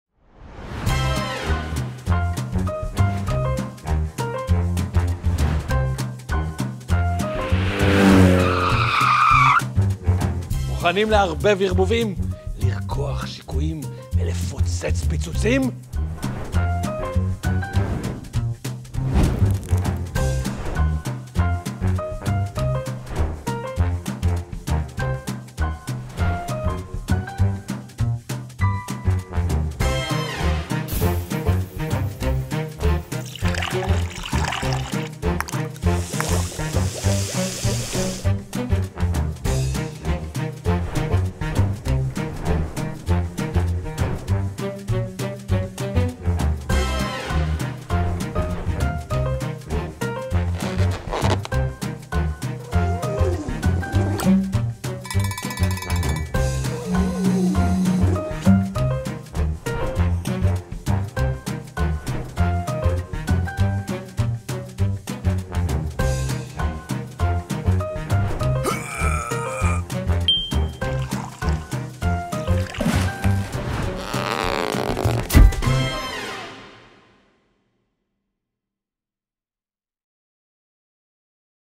אירוע סוכות במוזיאון
בדומה לטביעת אצבע, לכל אחד מאיתנו ישנו מאפיין ייחודי – מיקרוביוטה: עולם שלם של חיידקים ומיקרואורגניזמים נוספים המתארחים אצלנו בתוך מערכת העיכול. מוזיאון המדע מציג את "תחושת בטן", התערוכה שתיקח אתכם לסיור מאורגן במערכת העיכול שלכם.
מתאים לאנשים סקרנים, חובבי מדע וחֲפצי חיים בכל הגילים.

וזה עוד לא הכול! הכנו מגוון פעילויות לכל הגילים:

"מדע במטבח" מופע מבית היוצר של ד"ר מולקולה. כיצד הופכים את המטבח למעבדה ואת המעבדה למטבח? בעזרת חומרים שיש לכולנו בבית, המדען המטורף יערבב ערבובים, יעיף טילים, יפוצץ פיצוצים וירקח שיקויים. מופע בשיתוף קהל. בתוספת 5 ₪ למשתתף.

סביבת התנסות לקטנטנים: מותאם לבני 4–7.

"קיפולי בטן" – סדנת בנייה: יוצרים באמצעות קיפולי אוריגמי מערכת עיכול עם חותם אישי.

"קל לעיכול": הדגמה פעילה.

סיור מודרך לכל המשפחה בתערוכה.